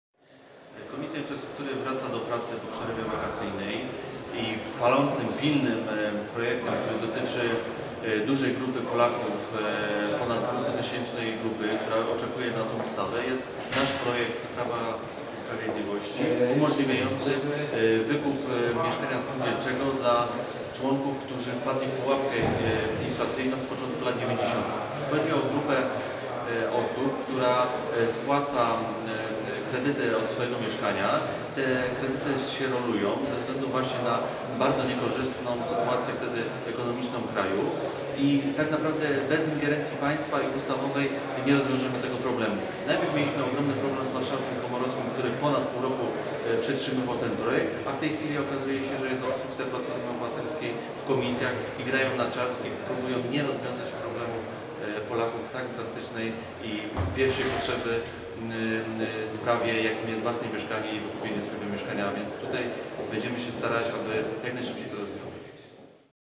Łukasz Zbonikowski - przedstawiciel PiS w Komisji Infrastruk
Łukasz Zbonikowski - przedstawiciel PiS w Komisji Infrastruktury

Komisja Infrastruktury powinna jak najszybciej zająć się problemem kredytów mieszkaniowych z początku lat 90-tych

Komisja Infrastruktury powinna w trybie pilnym zająć się problemem dotyczącym prawie 200 tysięcy Polaków. Chodzi o spiralę zadłużenia w jaką wpadły osoby zaciągające kredyty mieszkaniowe na początku lat 90-tych.